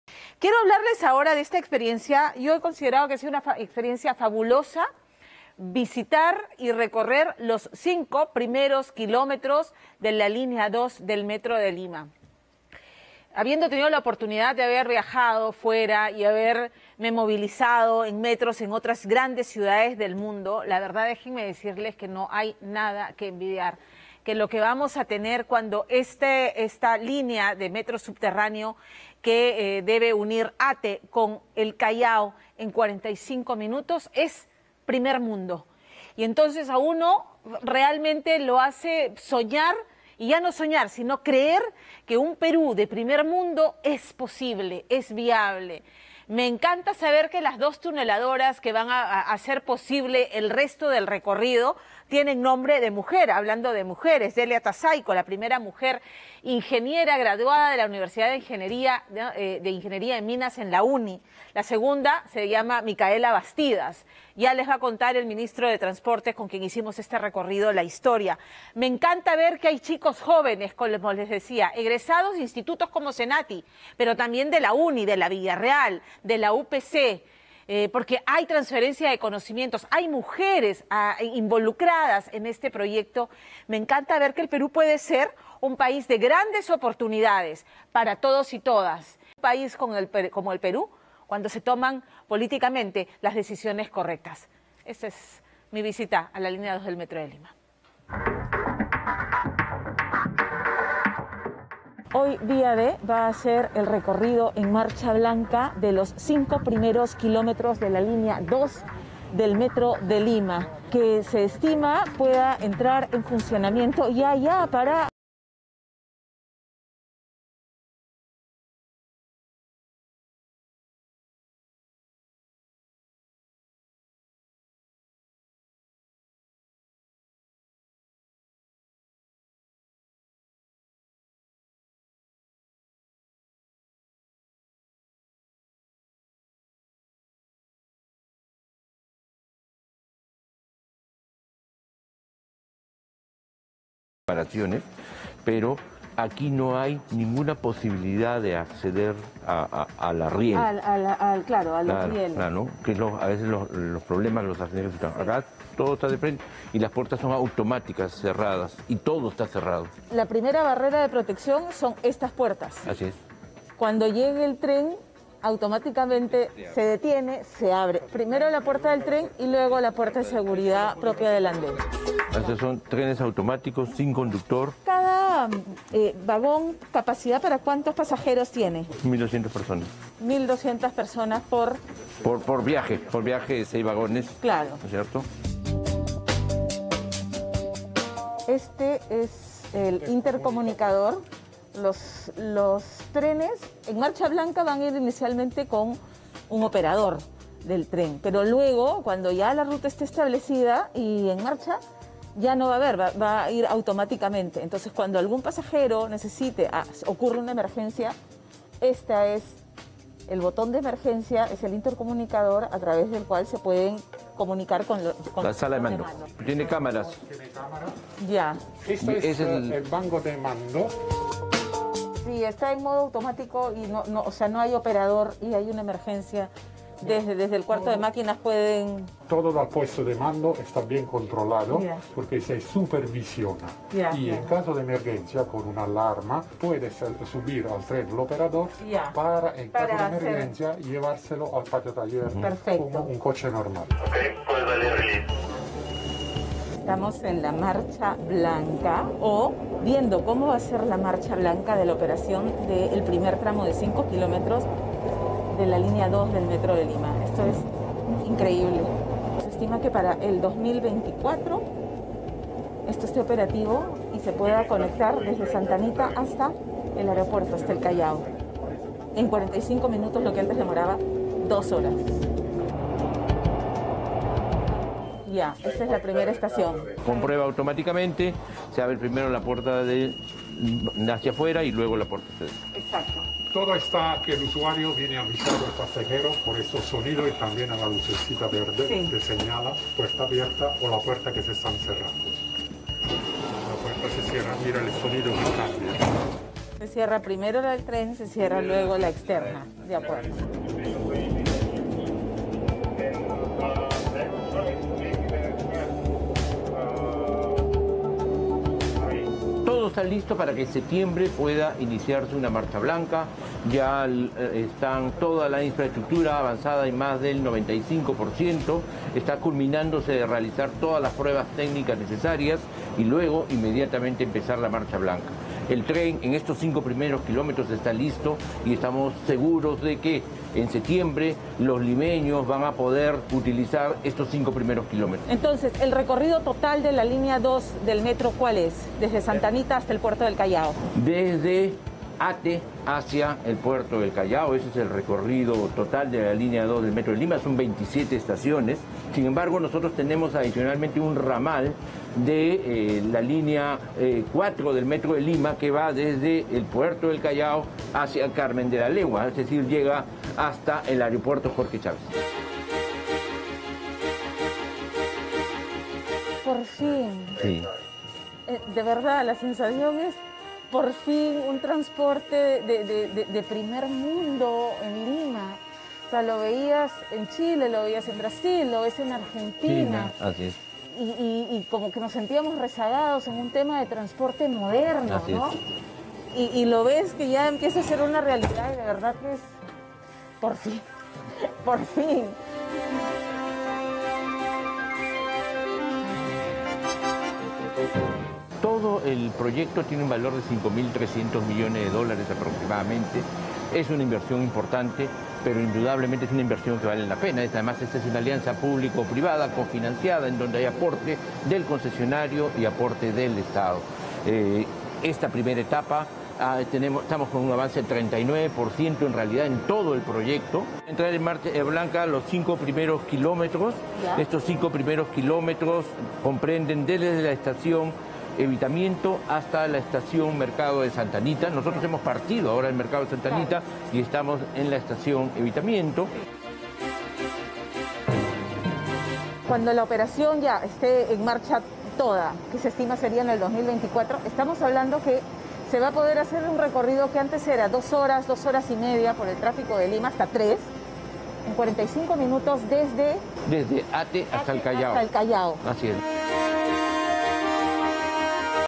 Un recorrido por los primeros 5 kilómetros de la Línea 2 del Metro de Lima
Día D recorrió junto al Ministro de Transporte los primeros cinco kilómetros de la Línea 2 del Metro de Lima. Unirá Ate con el Callao en 45 minutos. En septiembre iniciaría una marcha blanca, la infraestructura está avanzada en un 95%.